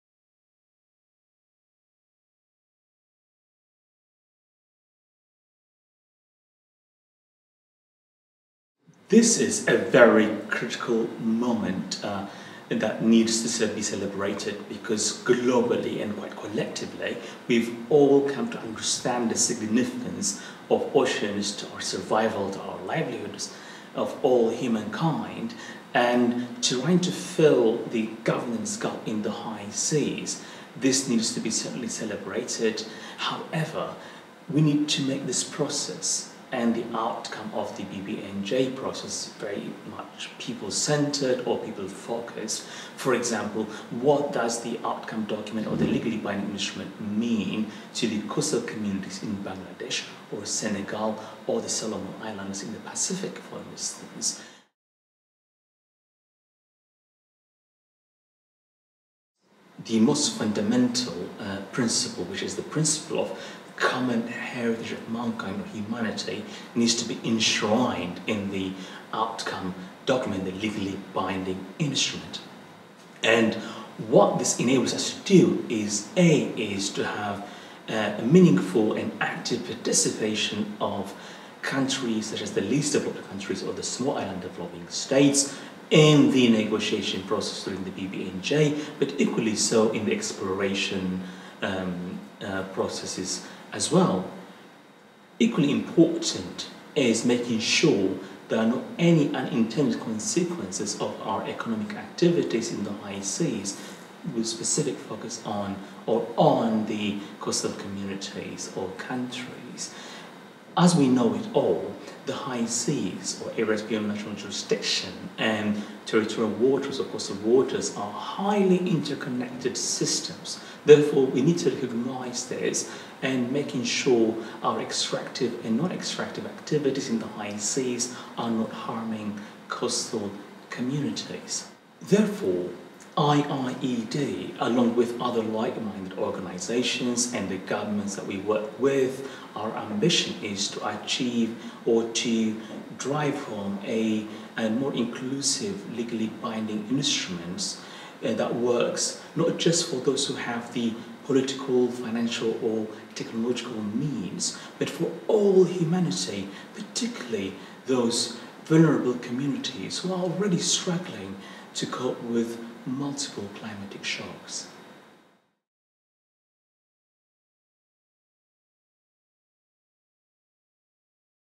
Why do we need fair and inclusive governance of the high seas?
An interview with Dr Essam Yassin Mohammed on why we need fair and inclusive governance of the high seas that is equitable and benefits all.

Mohammed, principal researcher at the International Institute for Environment and Development (IIED), also addresses how the institute is working with other organisations and governments to ensure measures are put in place so that 50% of the planet can be sustainably shared with 100% of the world's population.